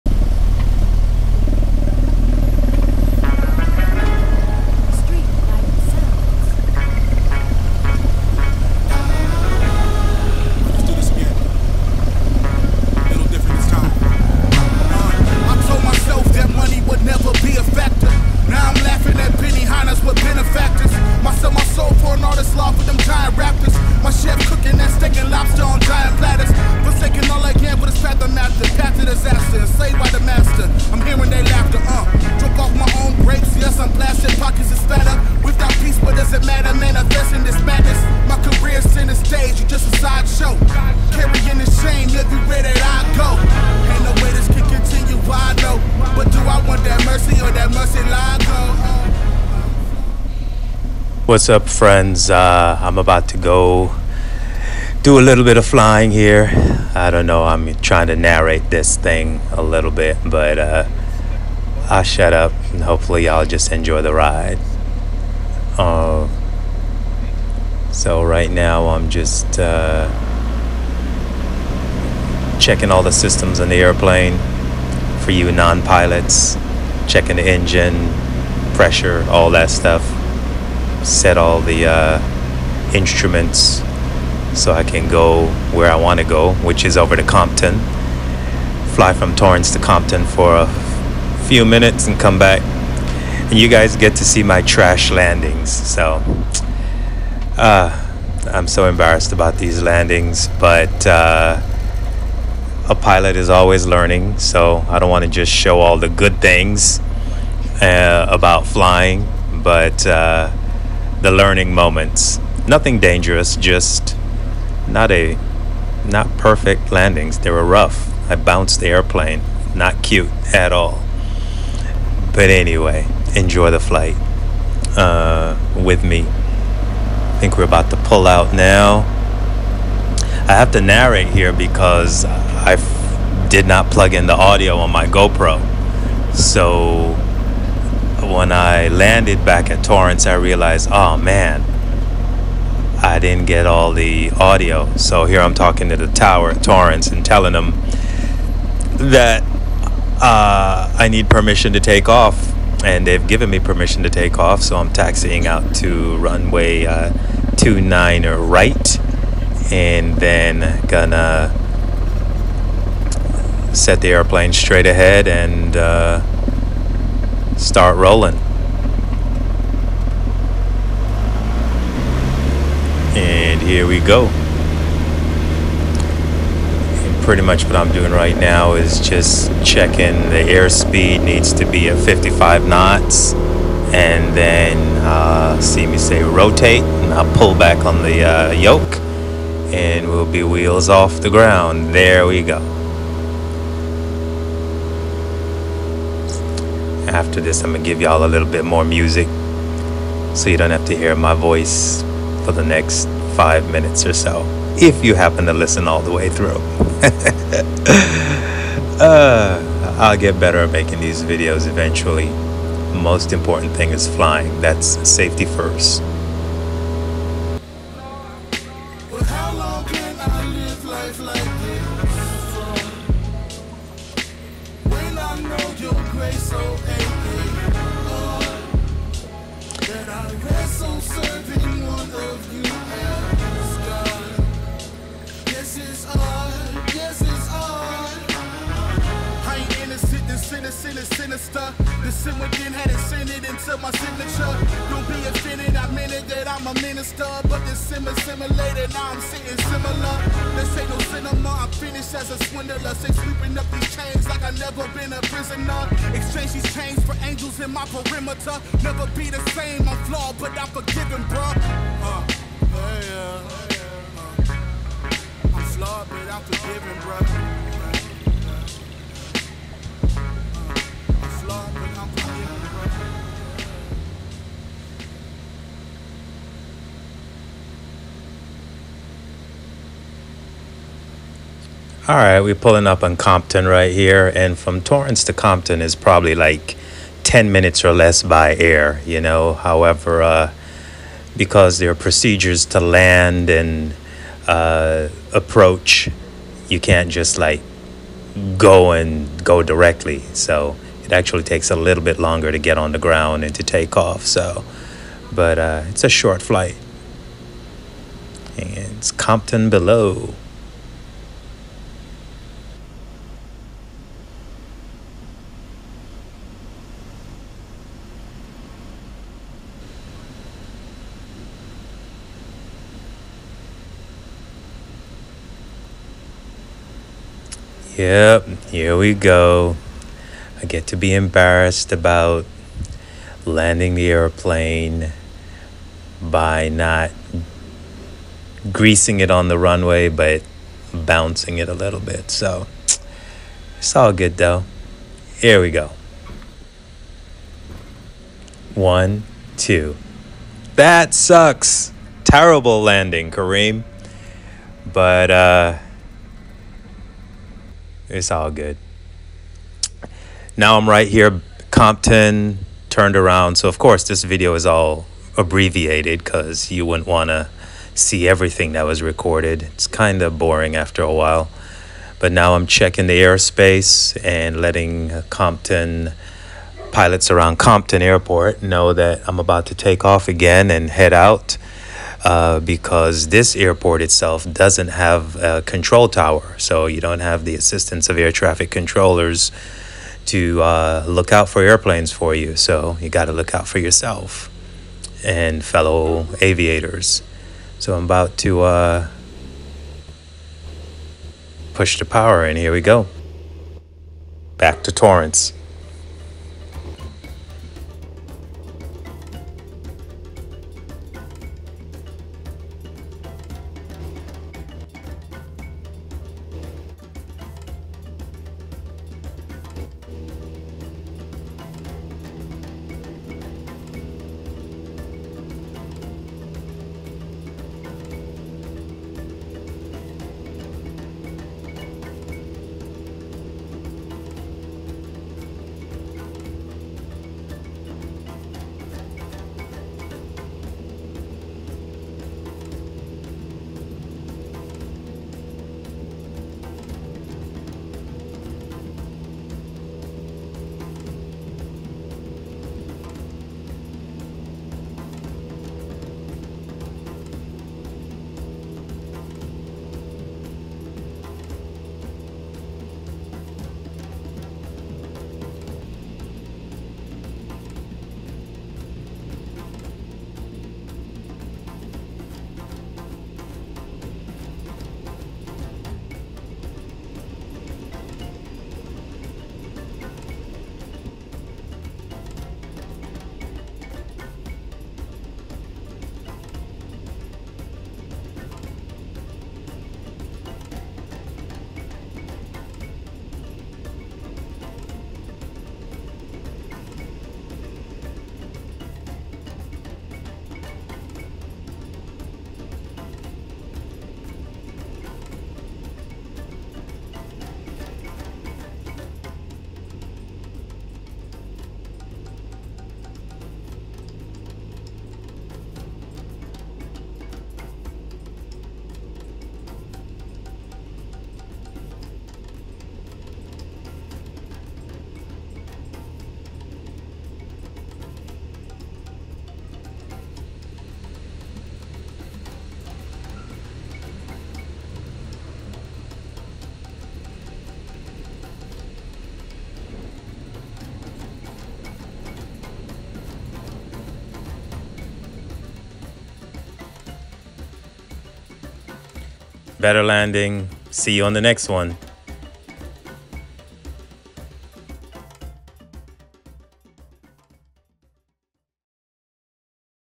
TORRANCE TO COMPTON
my first solo flight since passing my Instrument Rating Check ride. This was a VFR (Visual Flight Rules) mission. Quite a perspective having been under the "hood" for all the training and now I get to look outside again. LOL!!!